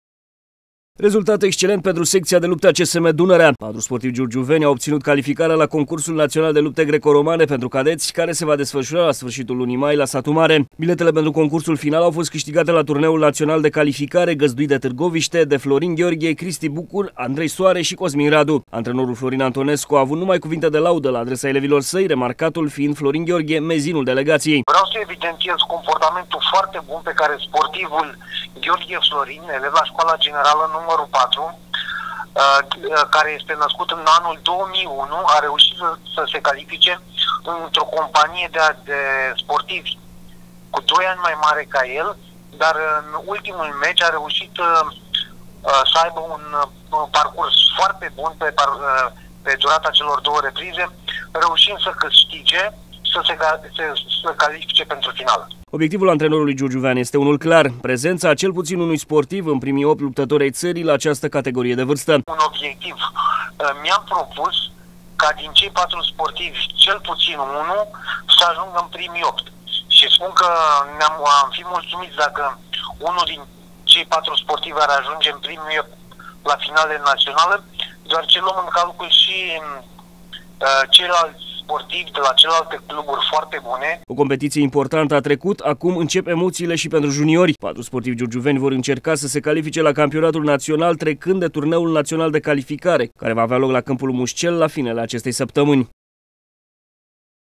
„CADEŢI” CU TALENT
O NOUĂ PERFORMANŢĂ PENTRU SPORTIVII DE LA SECŢIA DE LUPTE A CSM DUNĂREA. LA TURNEUL NAŢIONAL DE CALIFICARE PENTRU CADEŢI GĂZDUIT DE TÂRGOVIŞTE LA FINELE SĂPTĂMÂNII TRECUTE, PATRU GIURGIUVENI S-AU CALIFICAT PENTRU CONCURSUL FINAL.